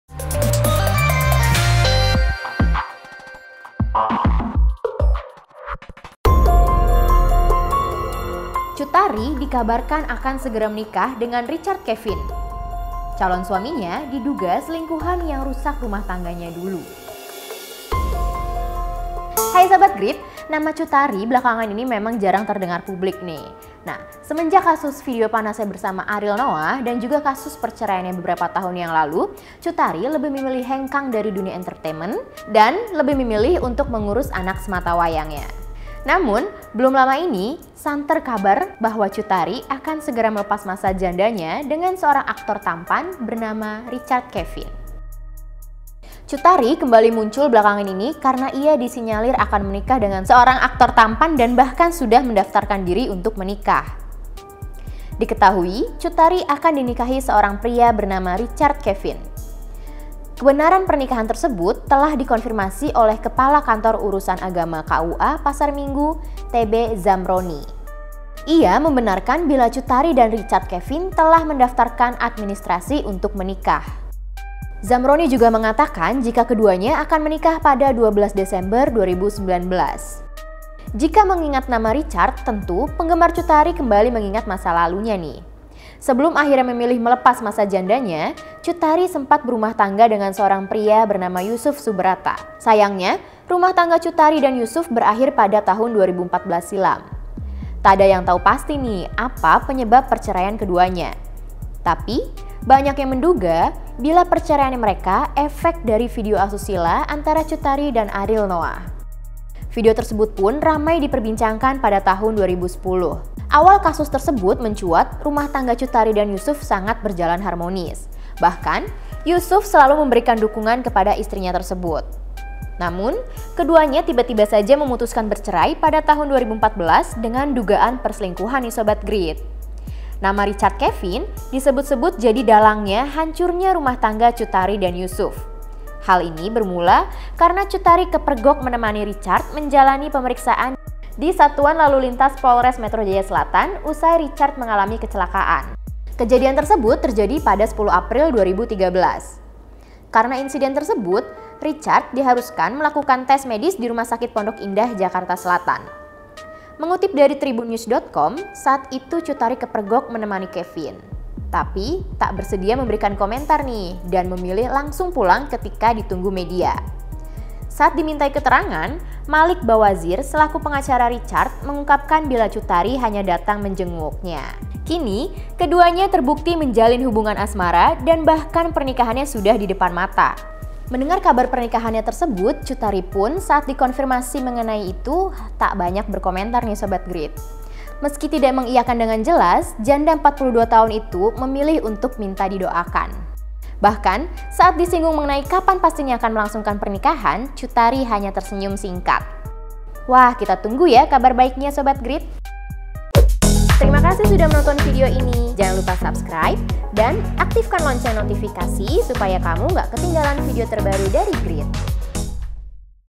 Cut Tari Akan Menikah Lagi, Calon Suami Diduga Selingkuhannya yang Dulu
Grid.ID - Nama Cut Tari belakangan ini memang sudah mulai jarang terdengar. Usai kasus video panas yang menyandungnya, juga kasus perceraian, Cut Tari memilih mundur dari dunia hiburan Tanah Air.

Cut Tari lantas memiliki kesibukan menjadi seorang ibu dari anak semata wayangnya.
 Cut Tari kembali muncul belakangan ini karena ia disinyalir akan menikah dengan seorang aktor tampan dan bahkan sudah mendaftarkan diri untuk menikah.

Diketahui, Cut Tari akan dinikahi seorang pria bernama Richard Kevin yang dahulu sempat diberitakan menjadi selingkuhannya.

(PM)

EDITORIAL OFFICE

ADVERTISING